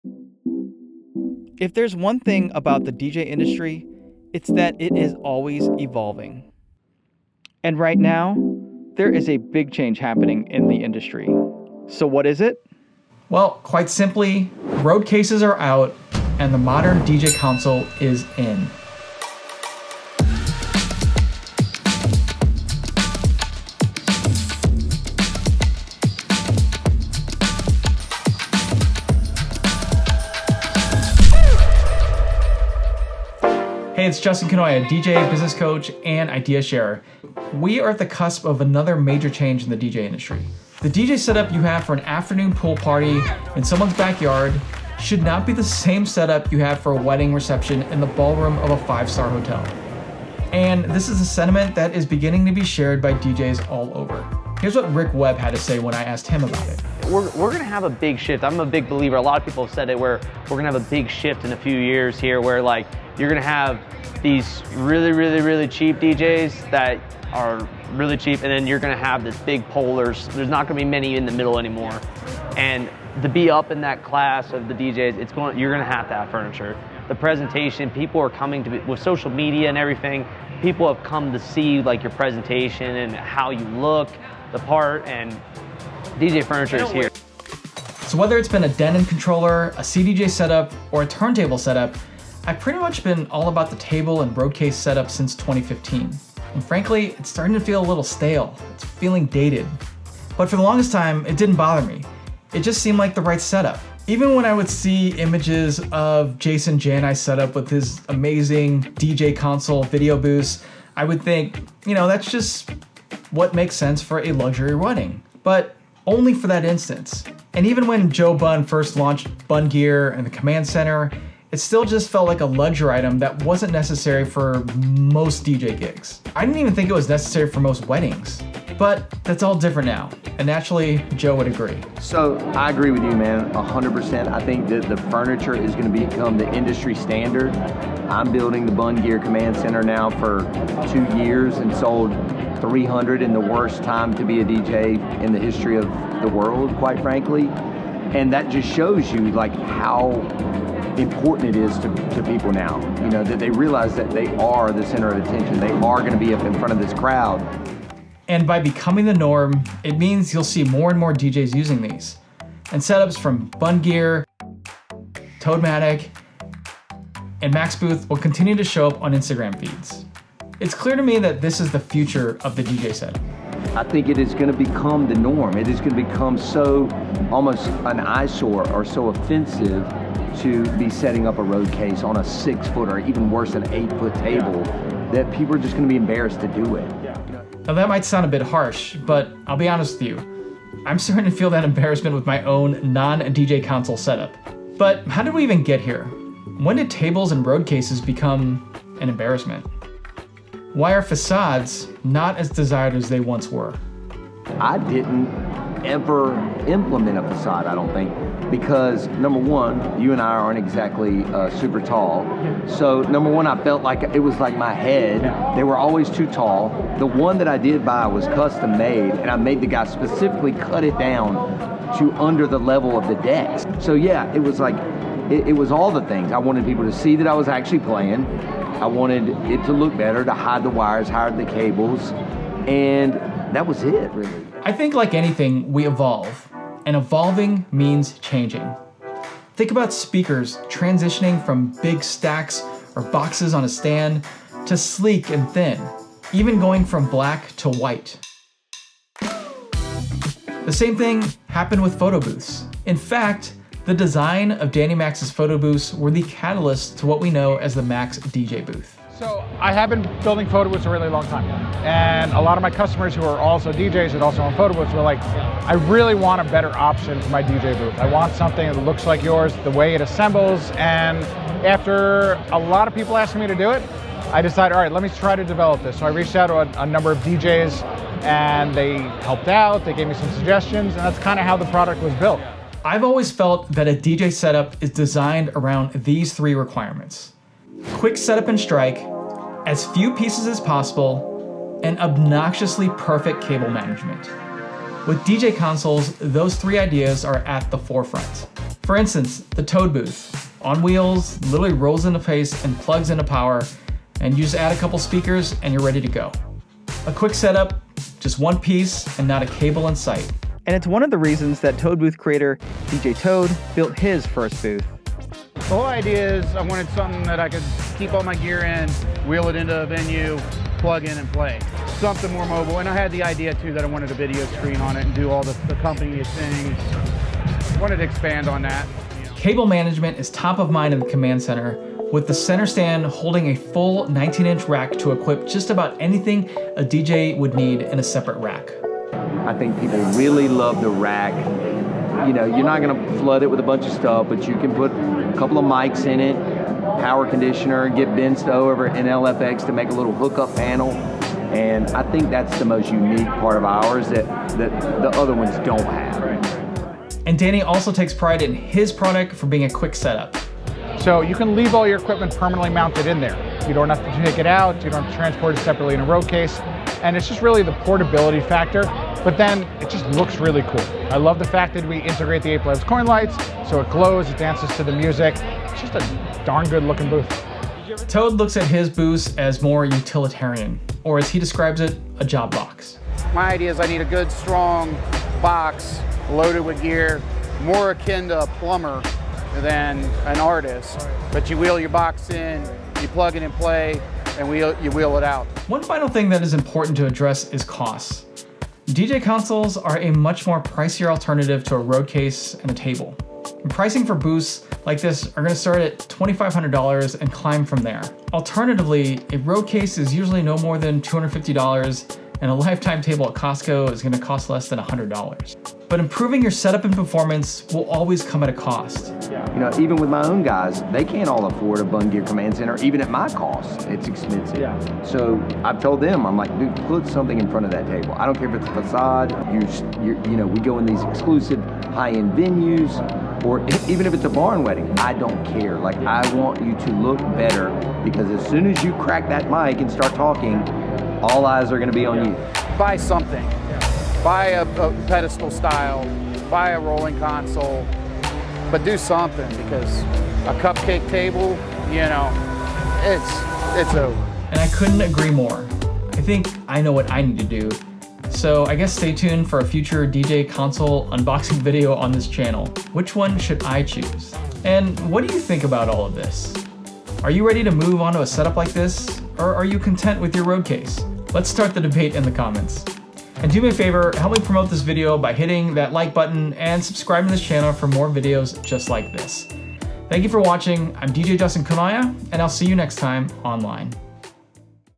Why DJs Are Ditching Their Roadcase For DJ Furniture
There's change a-coming in the DJ industry. Roadcases on a banquet table are so yesterday! In this video, I talked with Joe Bunn, Danny Max and DJ Toad about what inspired them to lead the charge in the new style of DJ consoles. While roadcases will always have a place in the industry, DJs are moving toward a completely different look when it comes to set-ups at weddings and other luxury events.
Learn more about the Bunn Command Center, Max DJ Booth and the Toamadtic rolling console.

DJ Kanoya Links:
Standing mat